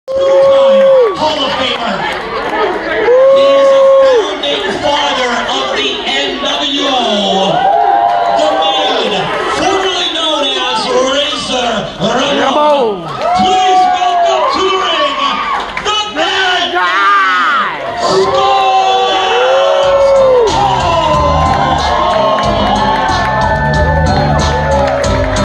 PWR Redemption(1)
These videos are from Redemption last night here in Erie, PA. We had a great time as this was my daughter's first time going. She can't wait to do it again. Enjoy all of the videos!